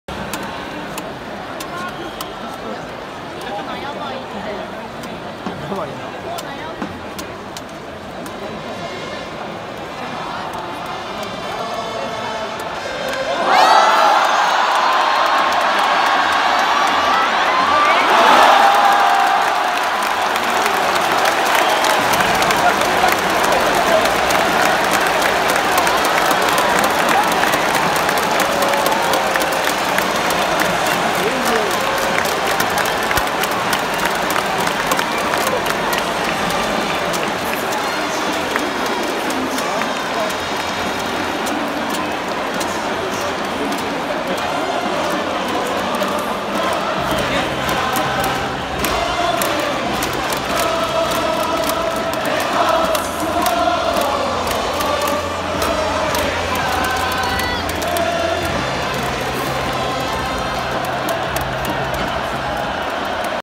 筒香選手 HR
2019/7/13
マイナビ・オールスターゲーム2019
オール・セントラル
VS
オール・パシフィック

次回予定
2019/7/25
阪神タイガース
VS